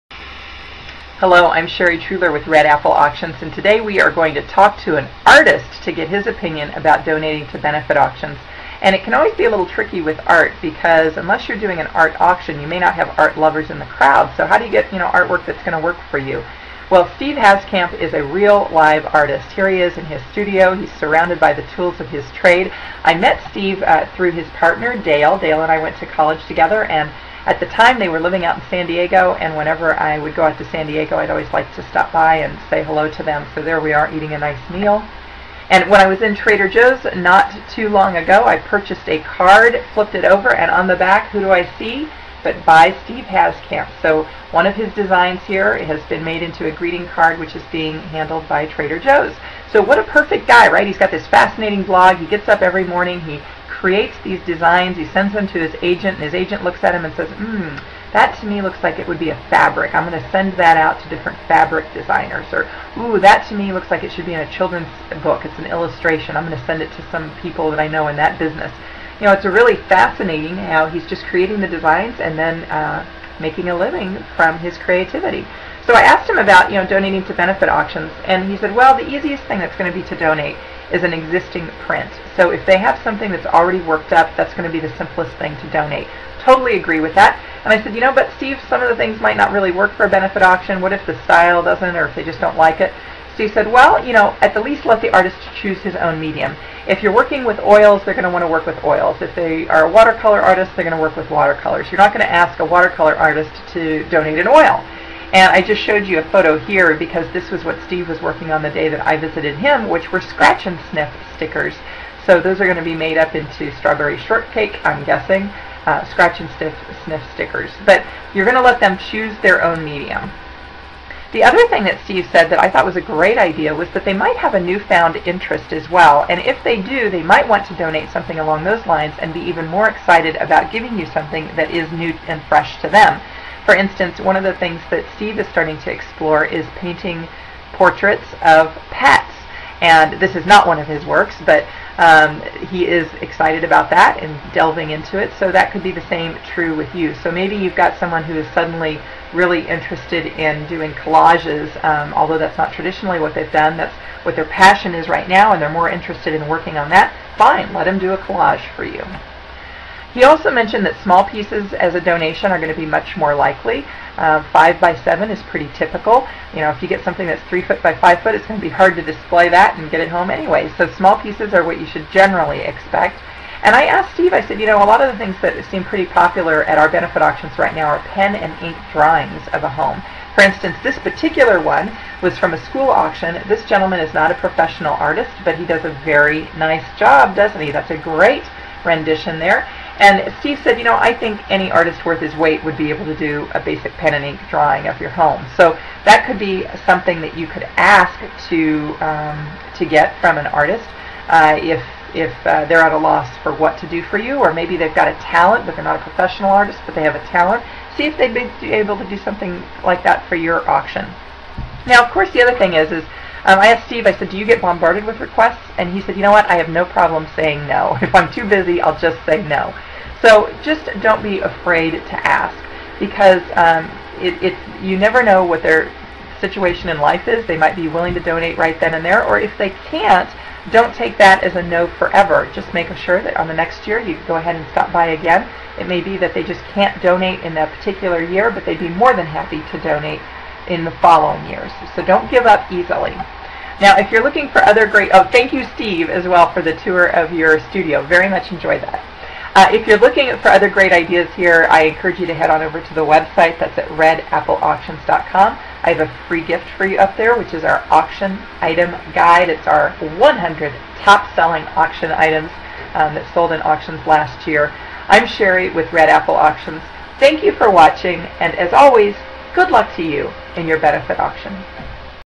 Benefit auction ideas: Asking the artist to donate (Steve Haskamp shares his perspective on ...
Meet Steve Haskamp. He's an honest-to-goodness, full-time artist. (He's the creative sort with cool auction ideas you should add to your benefit auction procurement wish list.)